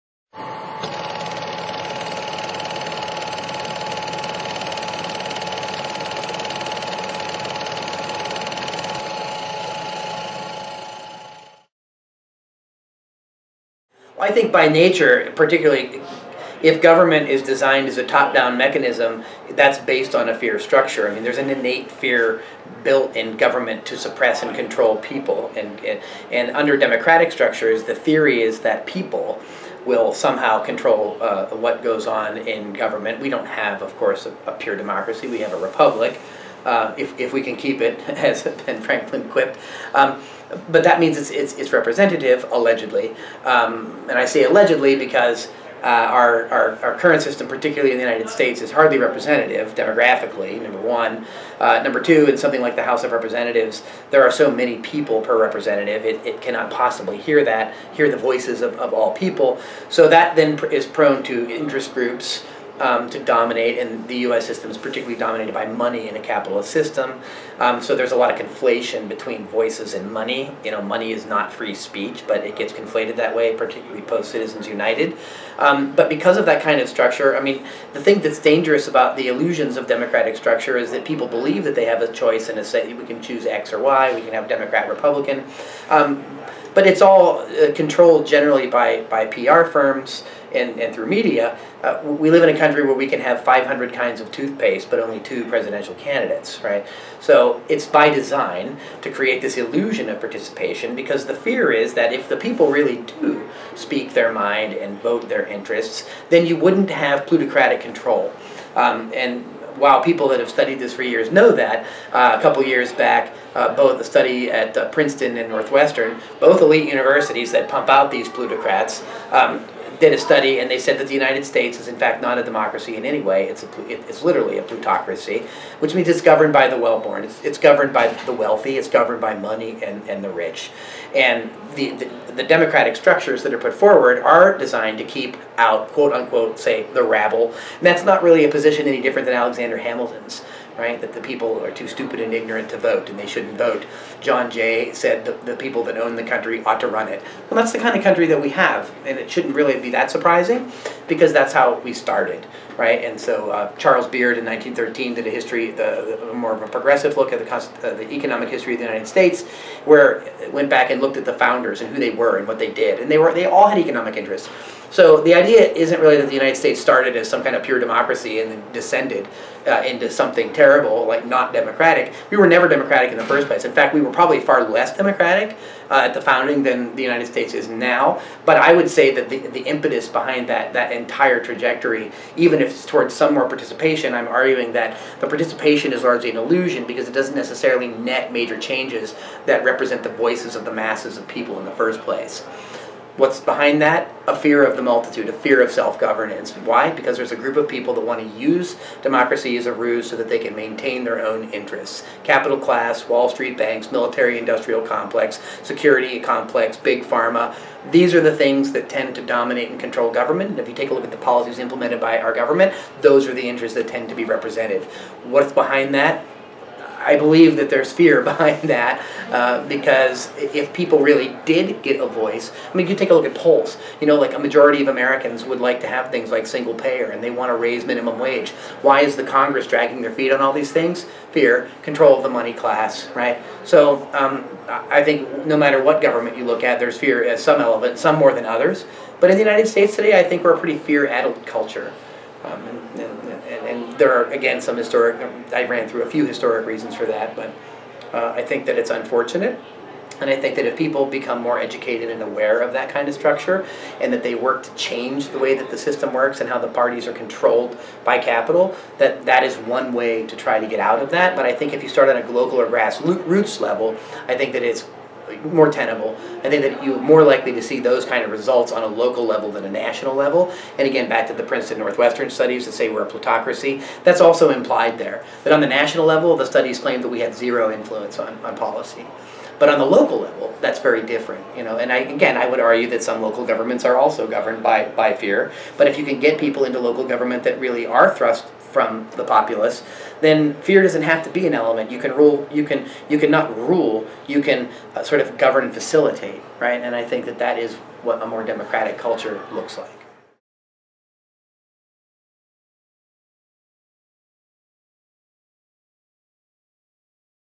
Mickey Huff - Why do so many governments fear their own people?
Mickey Huff, director of the independent media initiative Project Censored, spoke with the Weave at the Action in Media Education Summit in April 2016 at Sacred Heart University.